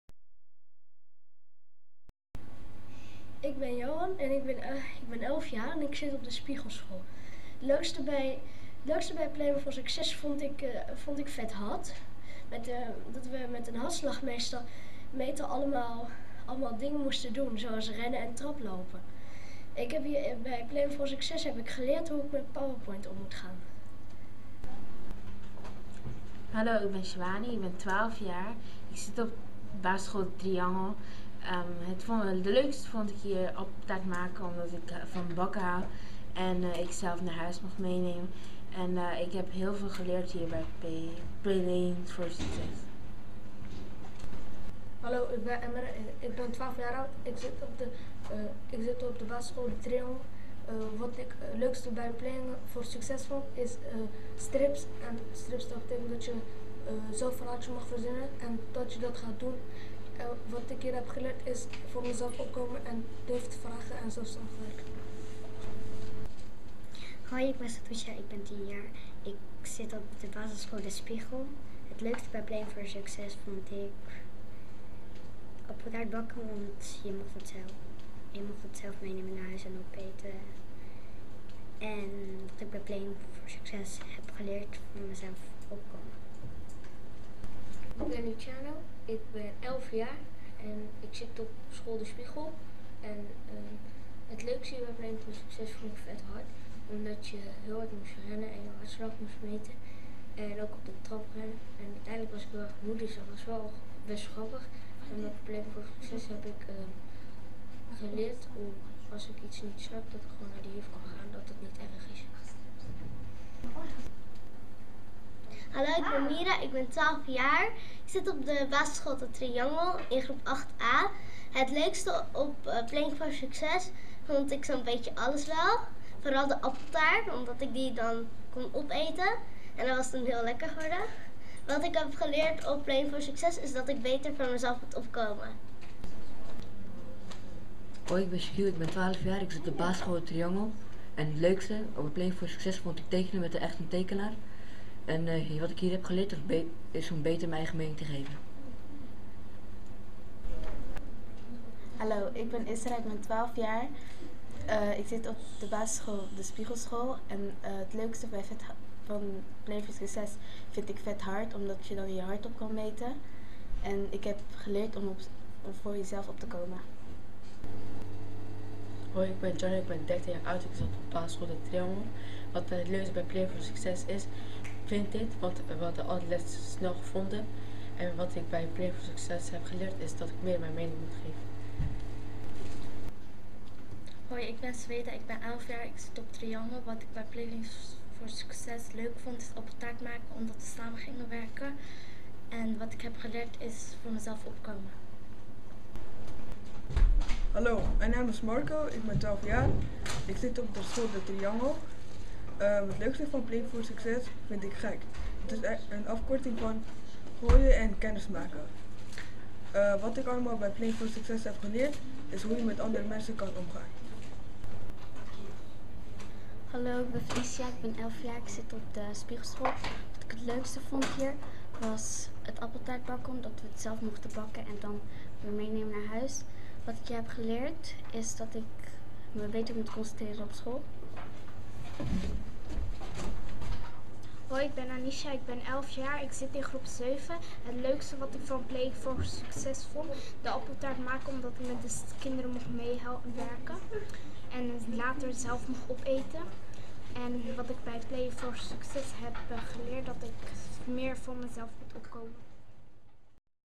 Eindstukjes groep 18
Wat hebben we de afgelopen 12 weken geleerd bij Playing for Success Den Haag? De kinderen van basisscholen 'Triangel' en 'De Spiegel' aan het woord!